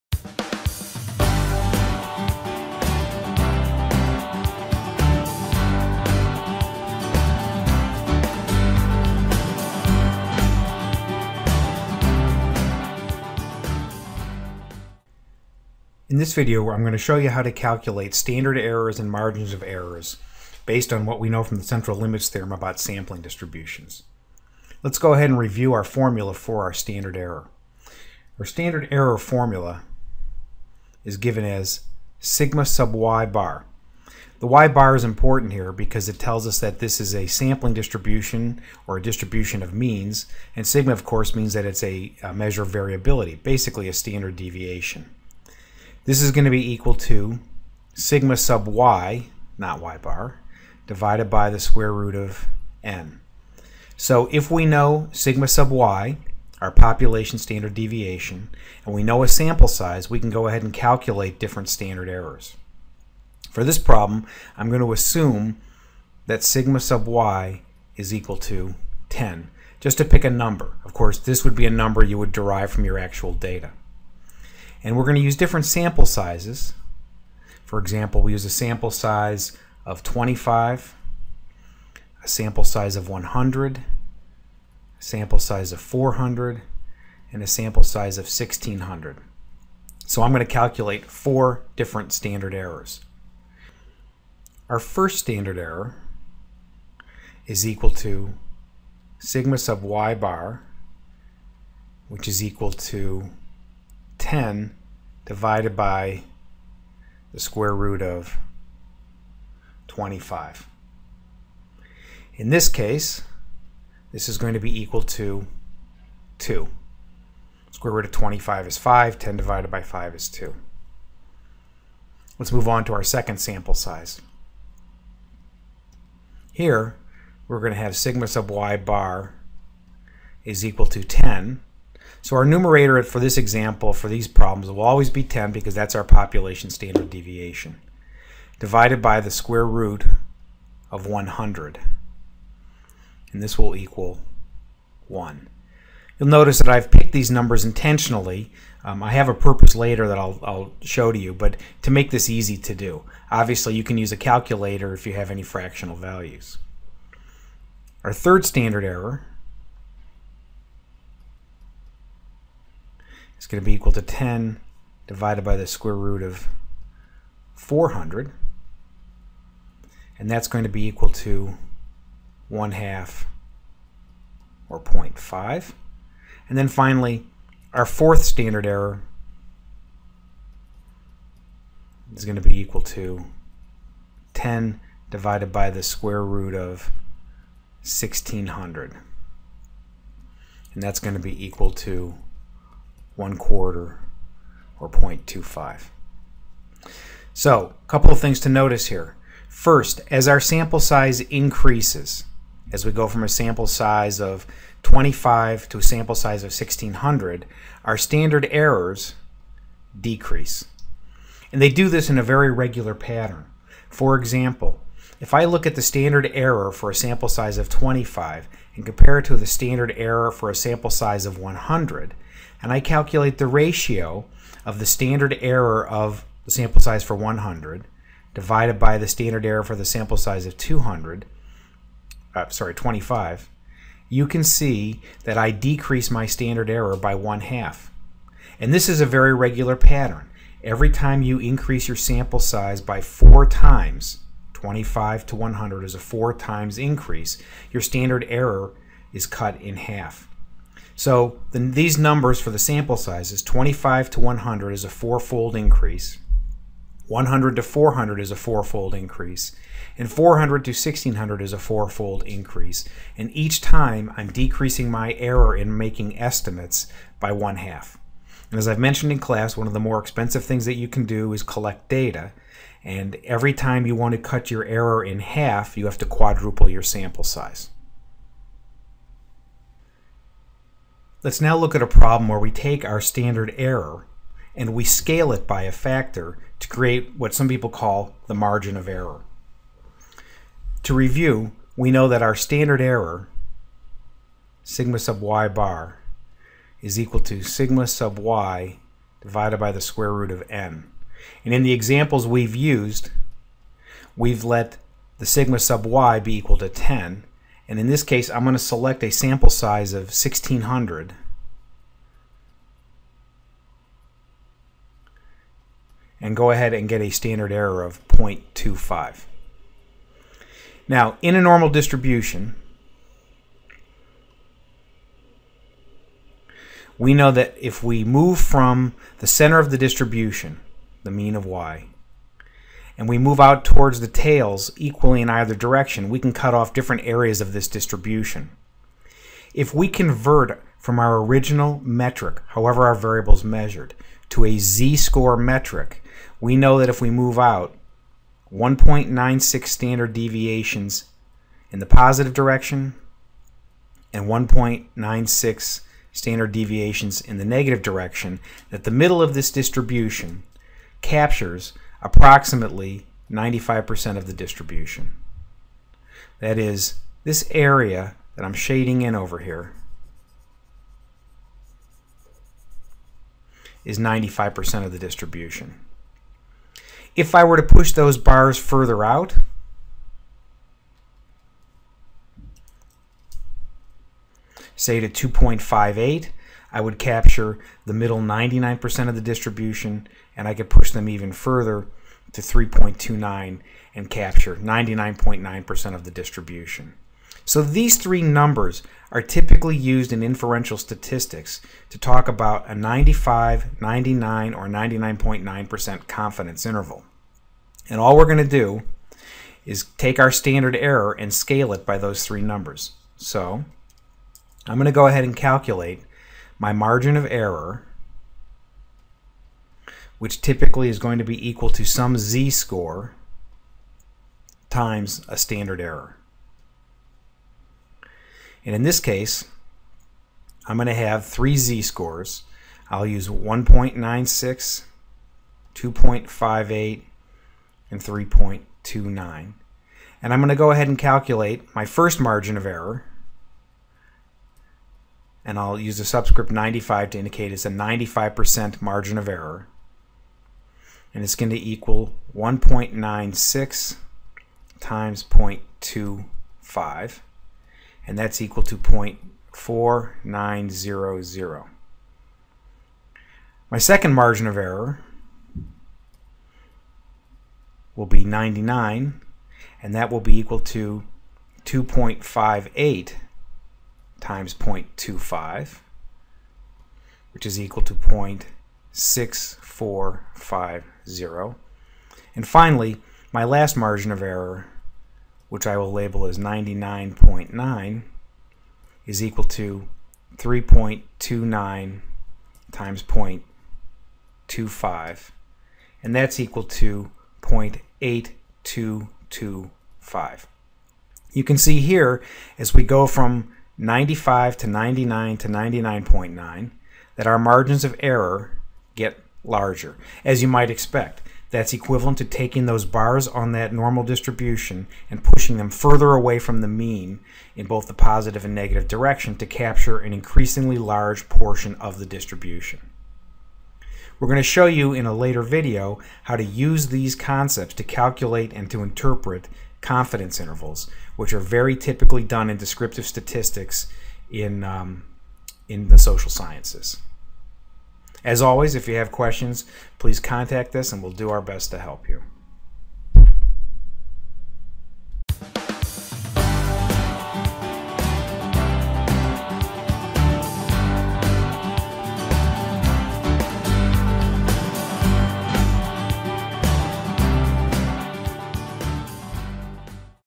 Calculating the Standard Error of a Mean and Confidence Intervals from Population Data
This video shows several examples of how to calculate a standard error for a mean of a population and a confidence interval.  The effect of sample size and z-statistic for the intervals are demonstrated.